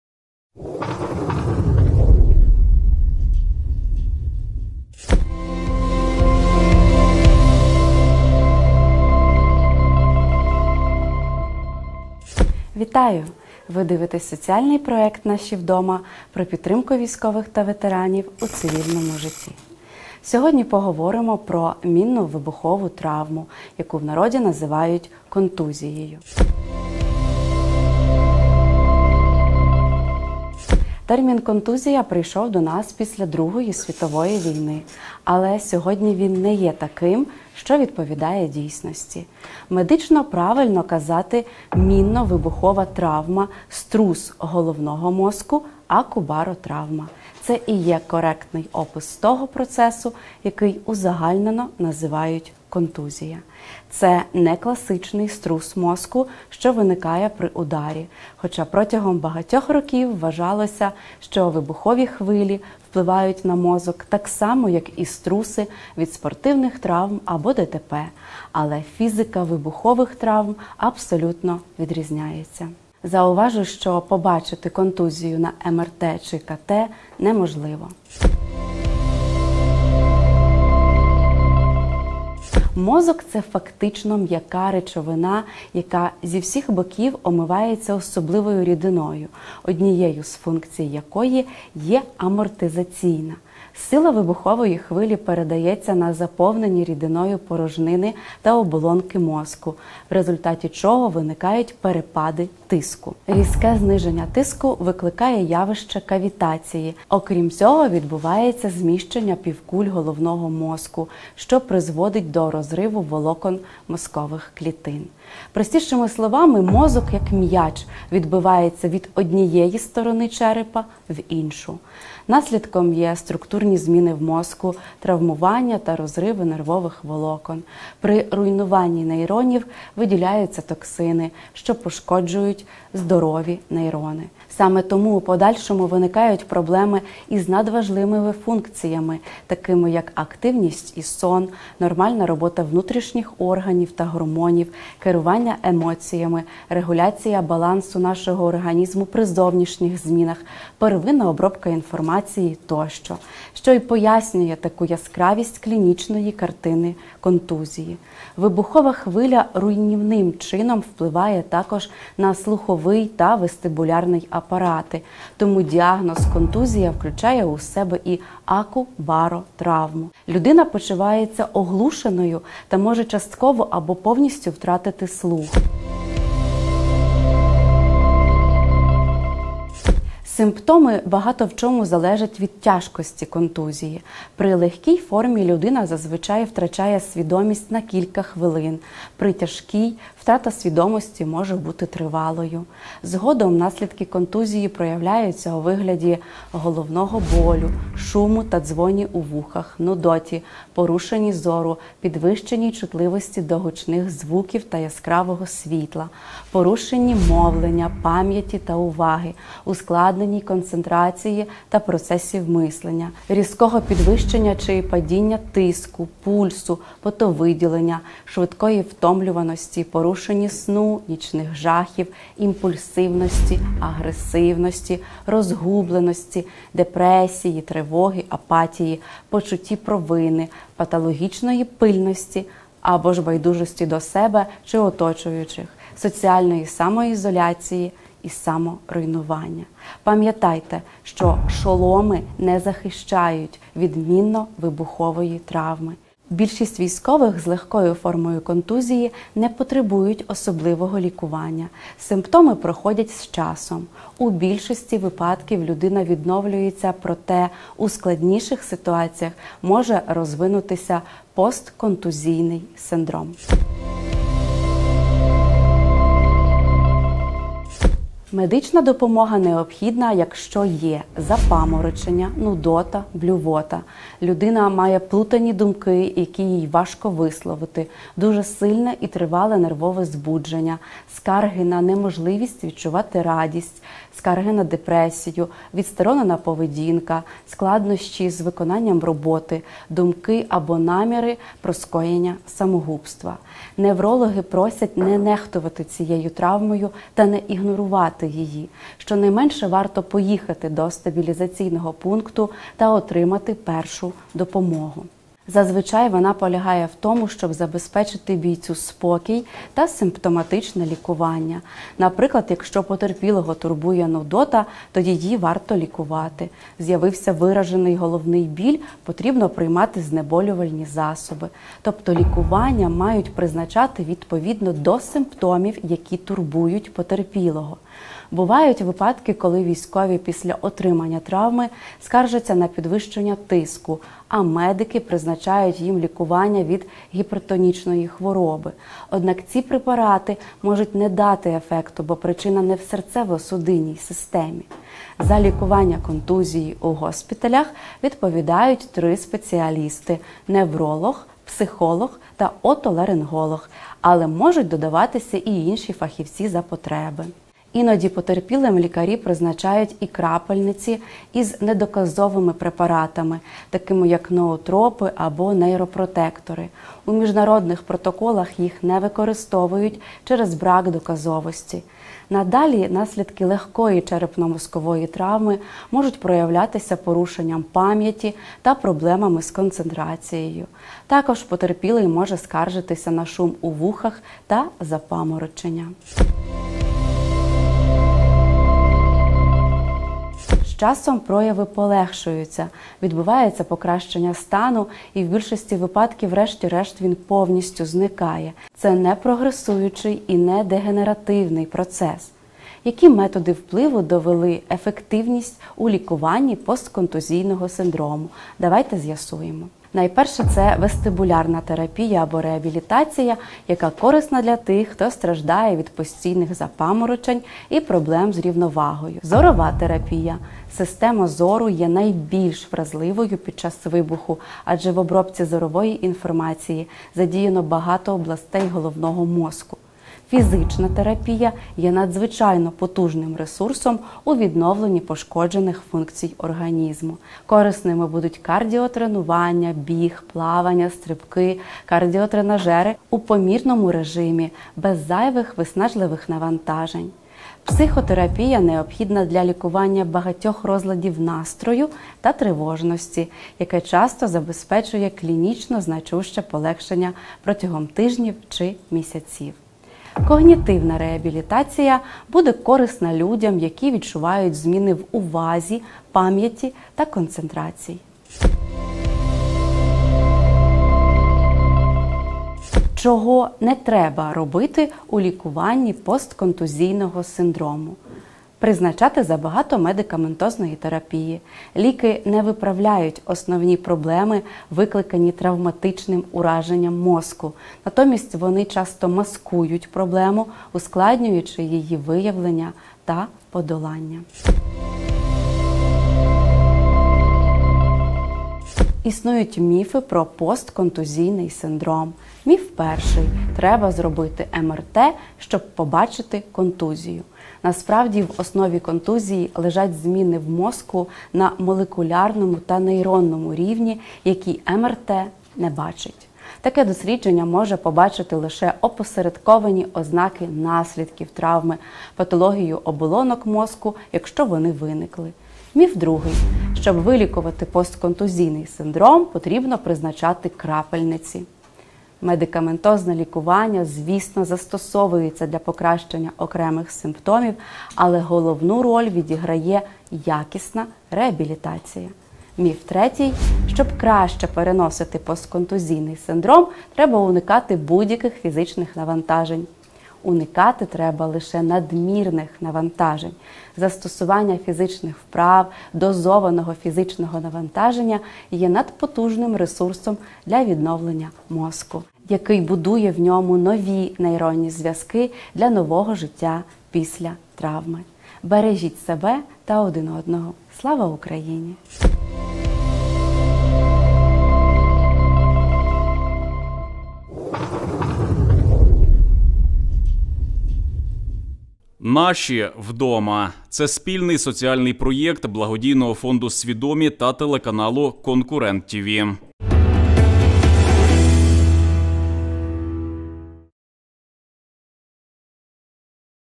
Наші вдома: Що таке контузія?
Медично правильно на контузію казати "Мінно-вибухова травма: струс головного мозку, акубаротравма". Це не "класичний" струс мозку, що виникає при ударі. Контузія не лікується крапельницями і її не можна побачити на МРТ.

00:00 - початок випуску
00:30 - що таке контузія?
01:22 - що відбувається з мозком?
04:25 - коли потрібна медична допомога?
06:30 - постконтузійний синдром
07:50 - чого не варто робити?
08:17 - міфи